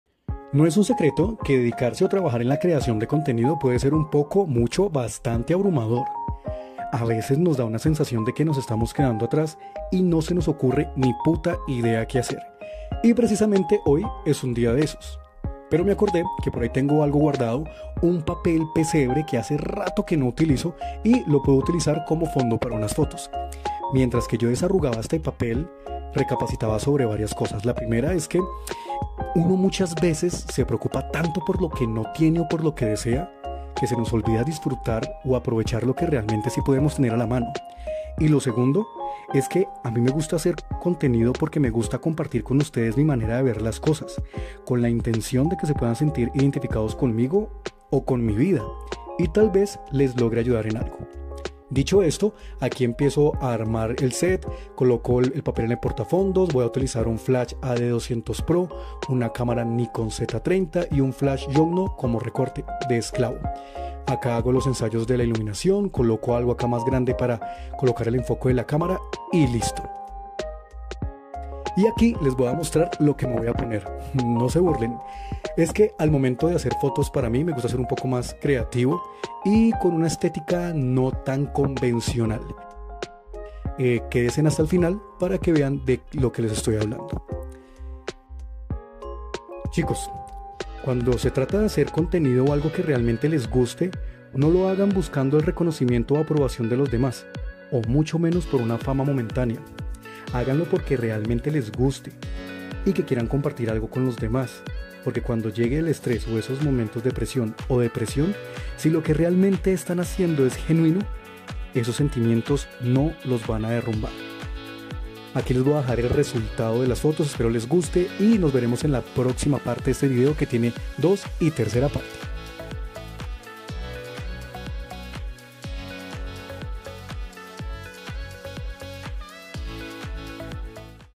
CREANDO CONTENIDO EN CASA PARTE 1
A veces nos sentimos abrumados por no saber que contenido producir, y más por la sobre saturación de creadores que hay actualmente.

Sin embargo podemos aprovechar lo que tenemos a la mano para generar cosas interesantes y originales